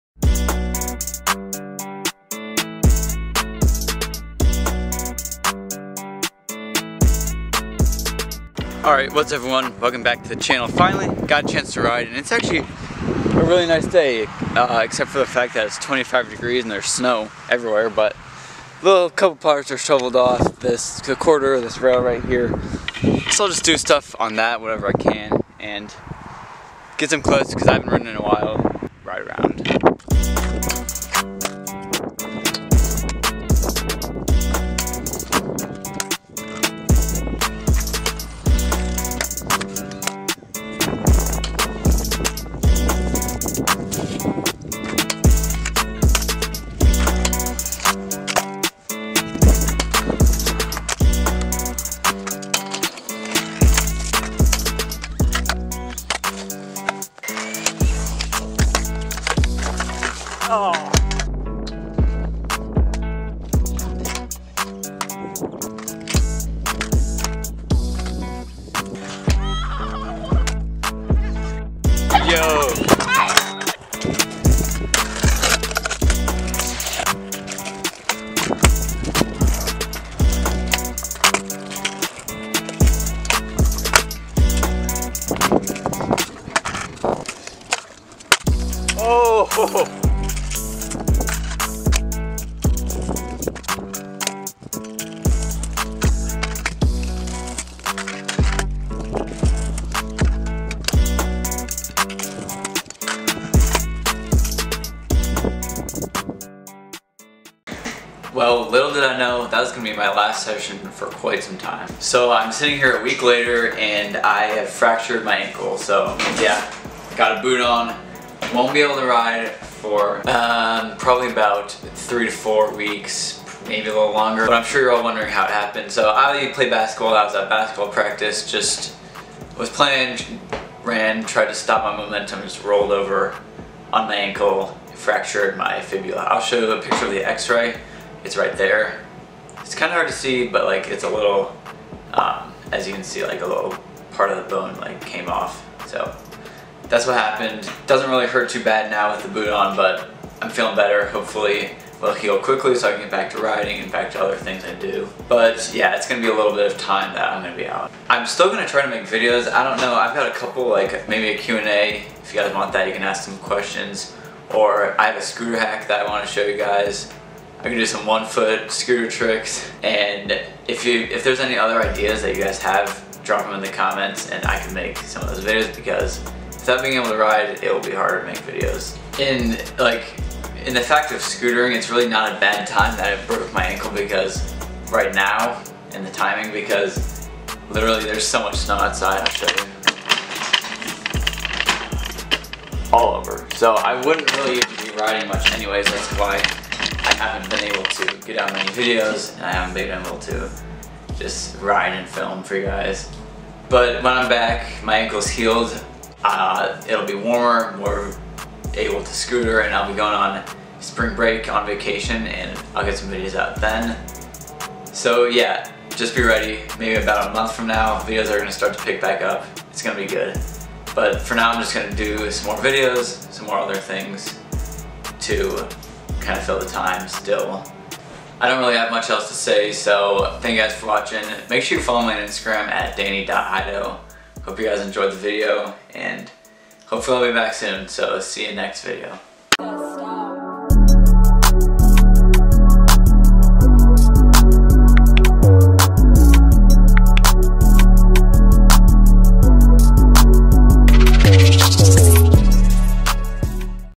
I BROKE MY ANKLE...
Well I guess I'll be out for a little. Fractured my ankle playing basketball and have a boot on for 3-4 weeks.